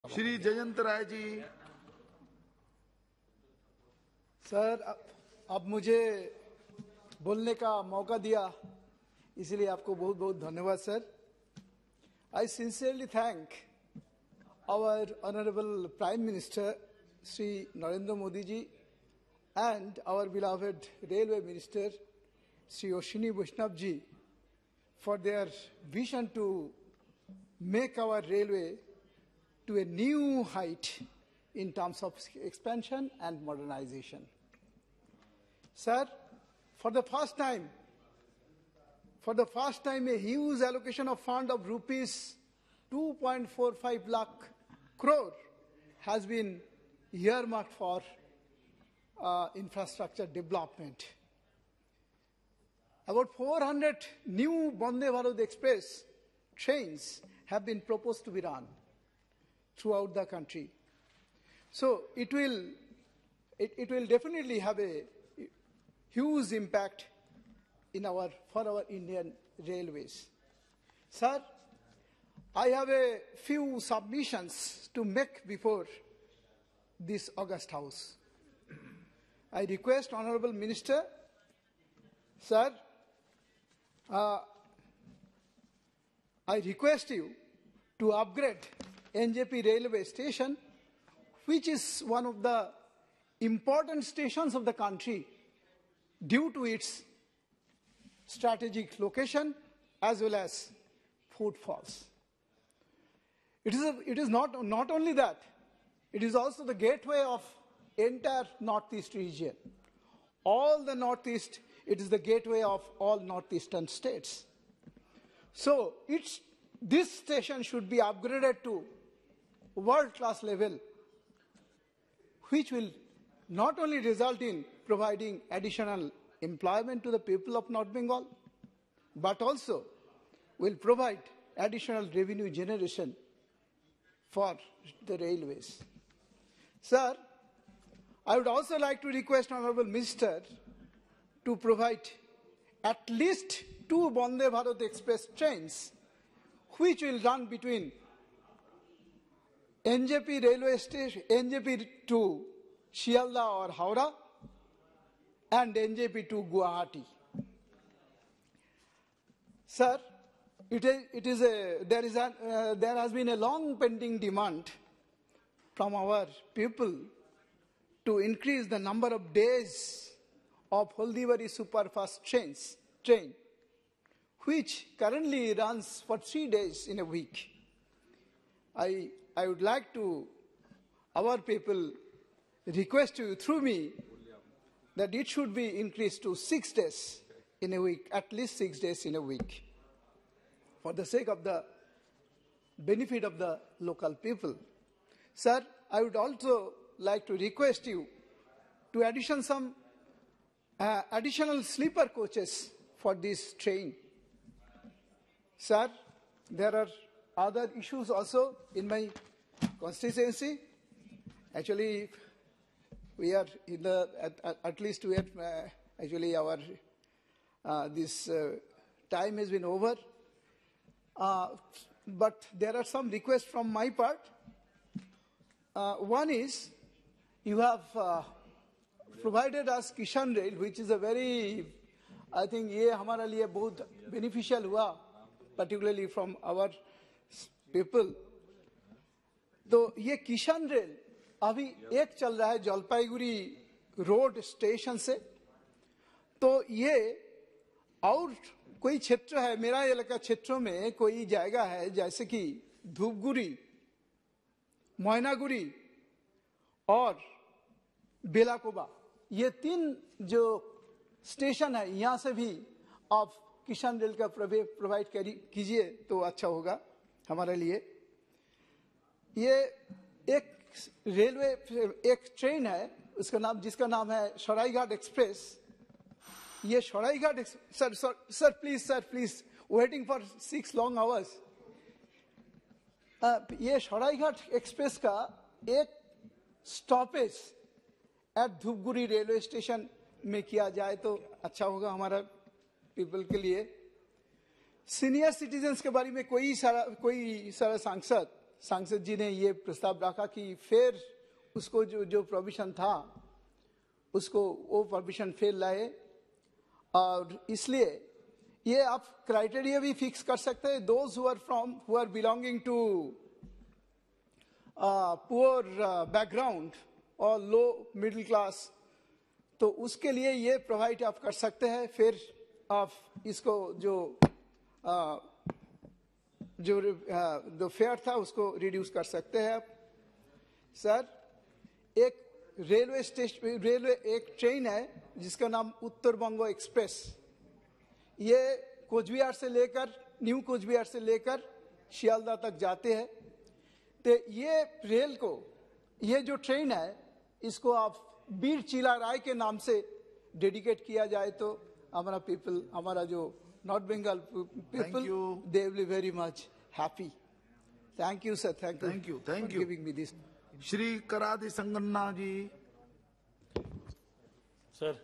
Dr. Jayanta Kumar Roy | Discussion on Demands for Grants under the control of Ministry of Railways
Sansad TV
Discussion on the Demands for Grants under the control of Ministry of Railways for 2022-23